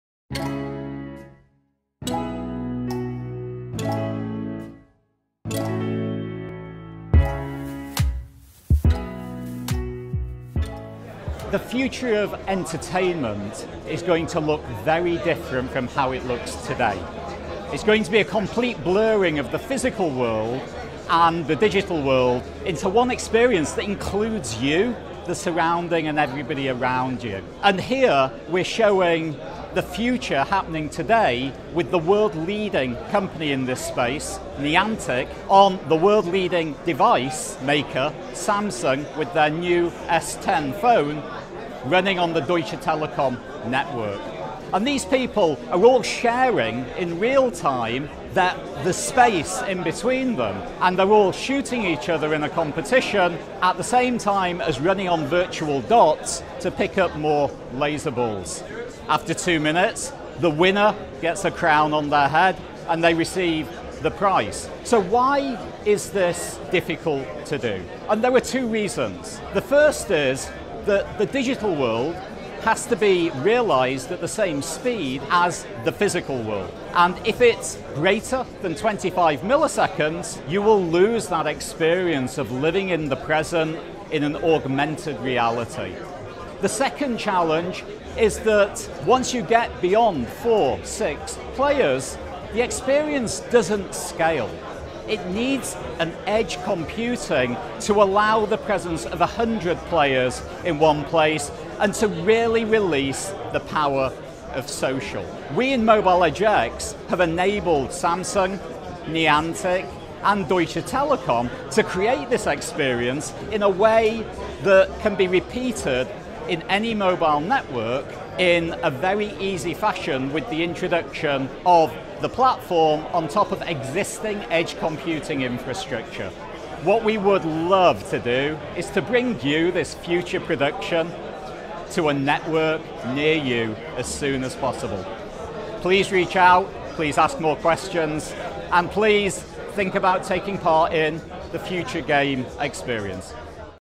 MobiledgeX: Enabling New Immersive Experiences With Edge Computing
MobiledgeX, Deutsche Telekom, Samsung and Niantic showcase the world's first edge-enabled, multiplayer augmented-reality demo at Mobile World Congress 2019. Geoff Hollingworth, CMO of MobiledgeX, explains how edge computing supports speed and ...